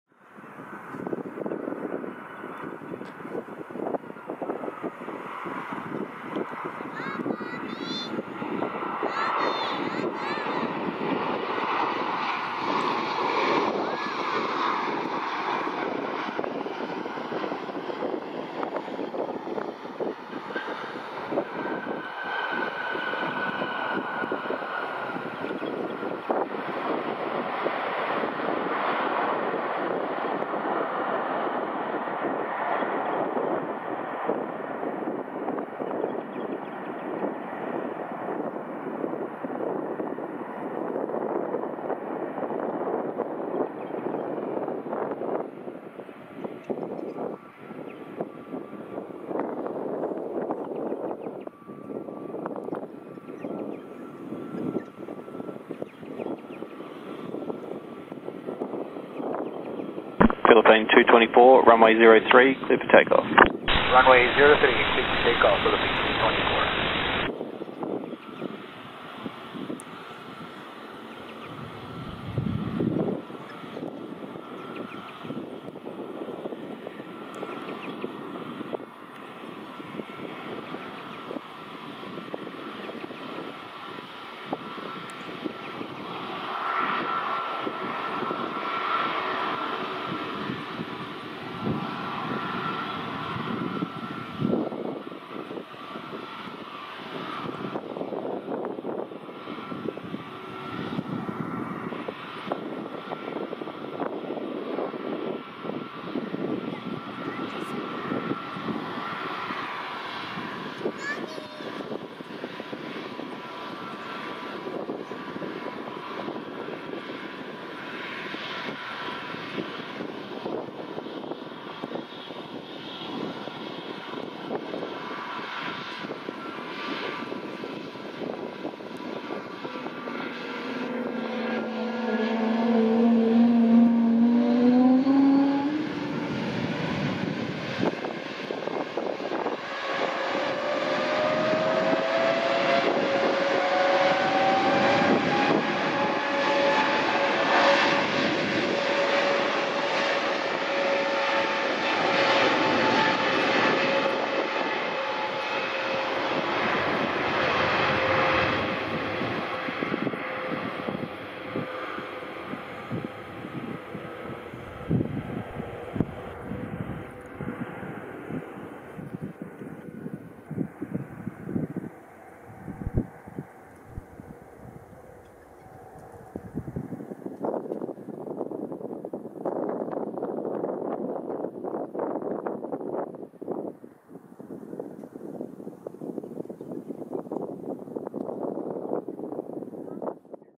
Philippine Airlines (RP-C9936) departs Perth Airport
In this video, One of Philippine Airlines' Airbus A321neos departs on Runway 03 at Perth Airport.

Information about this aircraft:
Registration: RP-C9936
Age: 5 Years
First Flight: 20 December 2018
Delivery date to Philippine Airlines: 31 December 2018.